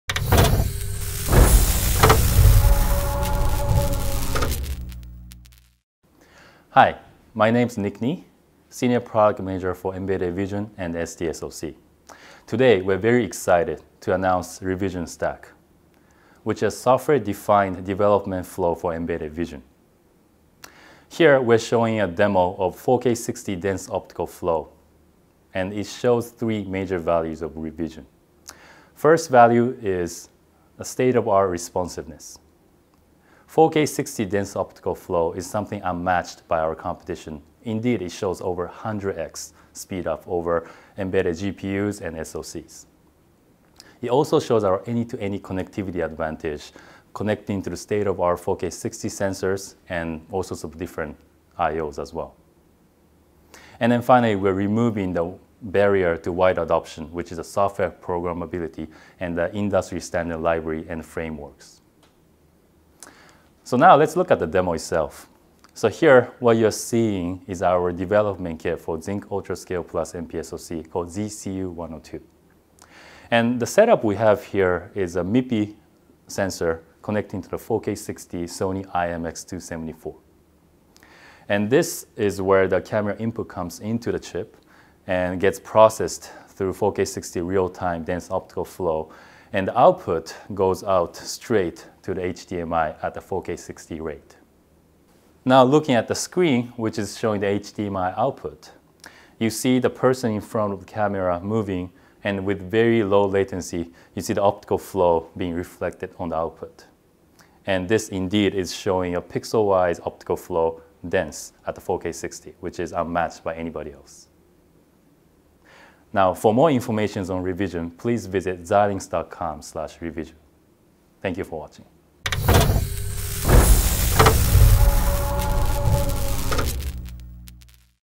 4K60 Dense Optical Flow Using the Xilinx reVISION Stack
This video demonstration shows the power of Zynq® UltraScale+™ MPSoC by running a state-of-the-art Dense Optical Flow algorithm at 4K resolution with 60 frames per second in the programmable logic.